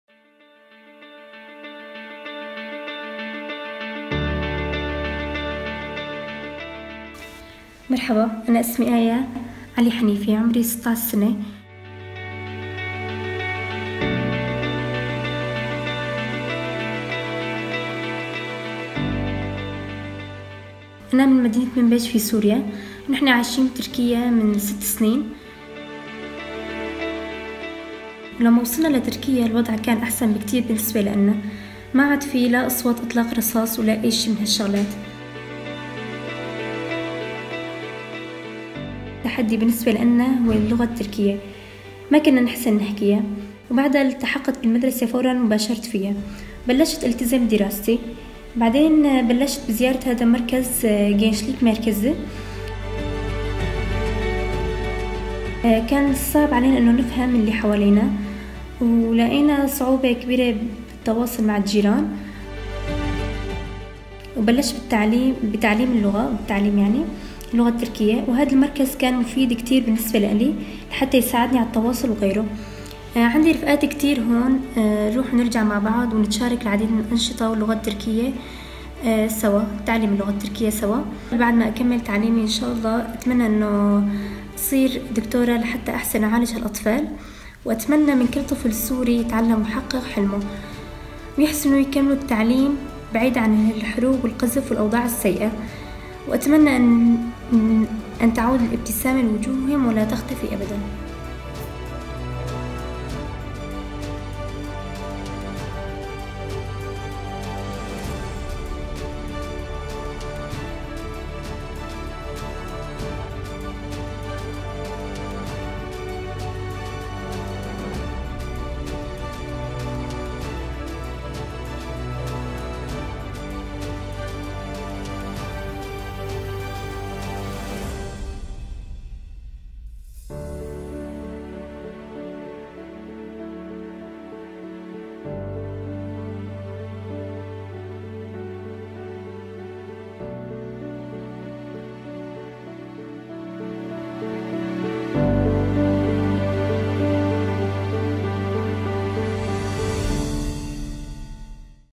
Giving Syrian refugee children a chance to realise their dreams
Aya is 16 years old. Having escaped the sound of bombs and gunfire in Syria, she has been in Turkey for almost six years. Aya was determined to finish her studies so took Turkish language classes. She hopes to become a doctor one day so that she can provide treatment to children. Aya’s language courses were part of the Non Formal Education Programme.

The Non Formal Education Programme is implemented in close collaboration between the Ministry of National Education, Ministry of Youth and Sports, Turkish Red Crescent, and UNICEF. The programme consists of three complementary education components designed for Syrian out-of-school refugee children or who are in-school but need extra educational support. With the financial support of the European Union, the programme aims to reach 65 000 Syrian children and adolescents across Turkey.